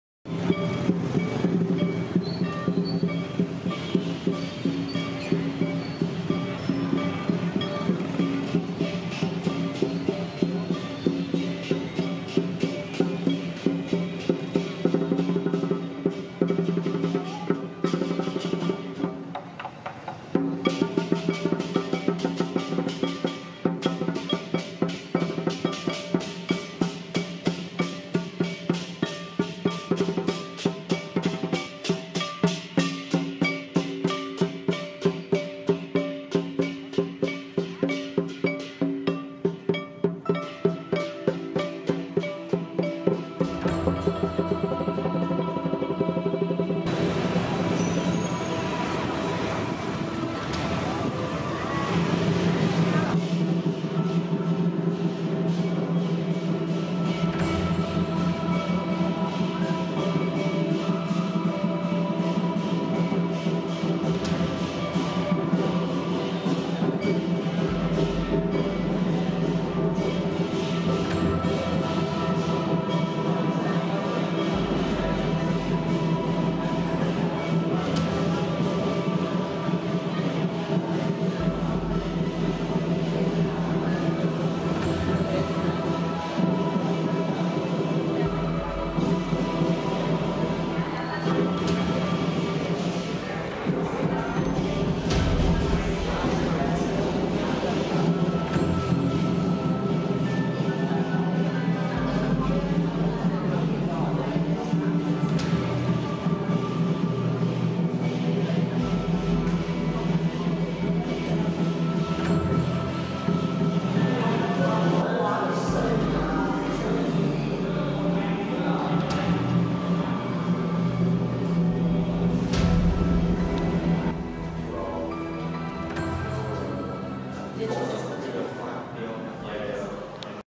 Drachenparade und Chinesischer Tempel in Saigon
Einmarsch von Drachentänzern zum chinesischen Tempel in Saigon, sowie ein kleiner Einblick in den Tempel.